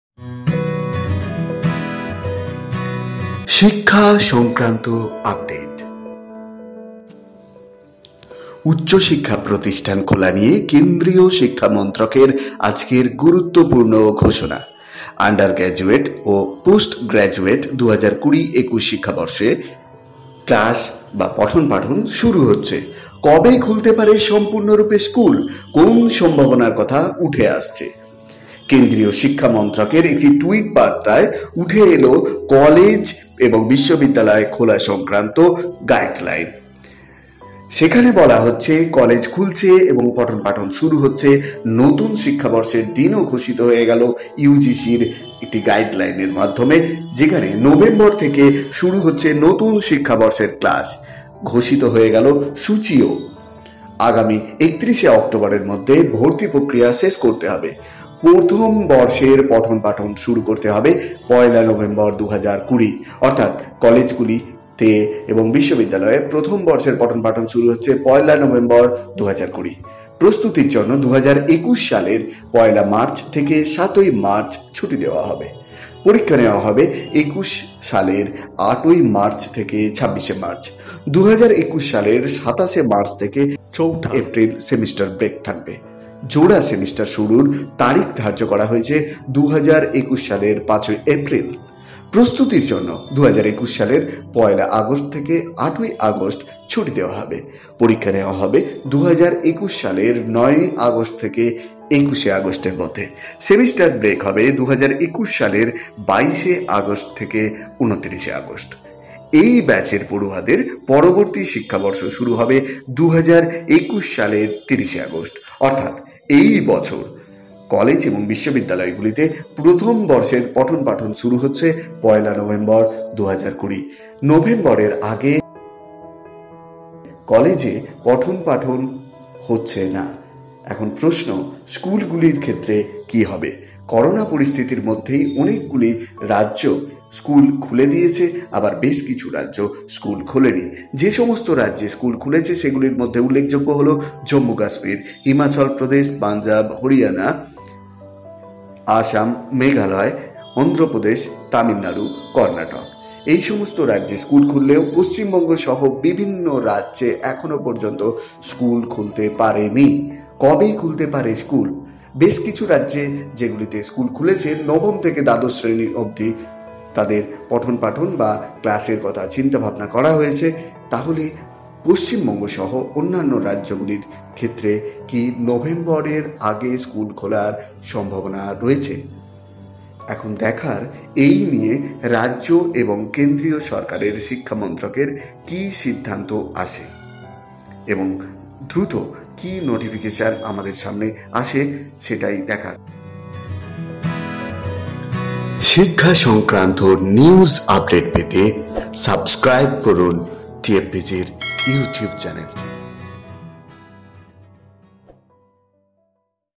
☞ Colleges reopen in November | কলেজ বিশ্ববিদ্যালয় খুলছে, স্কুল কবে?
UNIVERSITIES AND COLLEGES TO REOPEN FROM NOVEMBER, SAYS HRD AND UGC

করোনার জেরে  কলেজ- বিশ্ববিদ্যালয়ের ৯ মাসের শিক্ষাবর্ষ| অ্যাকাডেমিক ক্যালেন্ডার বেঁধে দিল ইউজিসি| সিমেস্টার হবে মার্চ এবং অগাস্ট মাসে| পয়লা নভেম্বর থেকে কলেজে শুরু হবে নতুন শিক্ষাবর্ষ| ৩১শে অক্টোবরের মধ্যে শেষ করতে হবে কলেজে ভর্তির প্রক্রিয়া|